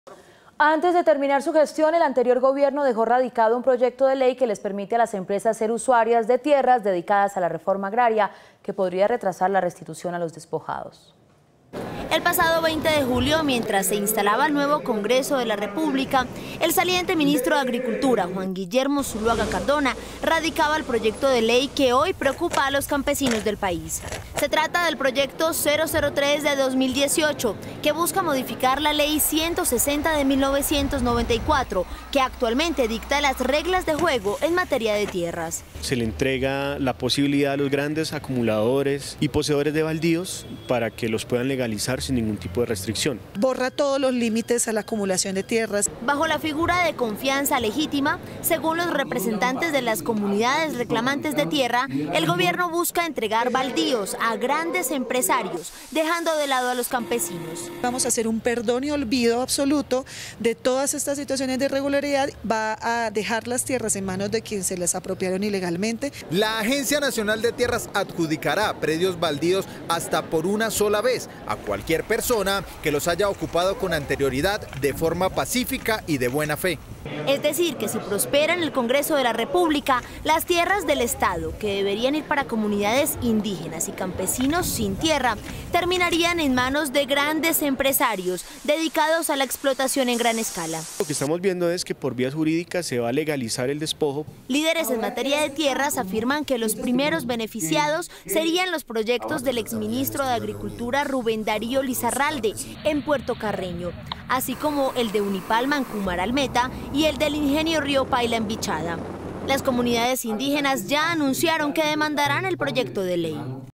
Gobierno Santos dejó radicado proyecto de ley que retrasaría restitución de tierras a despojados
Antes de terminar su gestión, el anterior gobierno dejó radicado un proyecto de ley que les permite a las empresas ser usuarias de tierras dedicadas a la Reforma agraria, que podría retrasar la restitución de tierras a los despojados.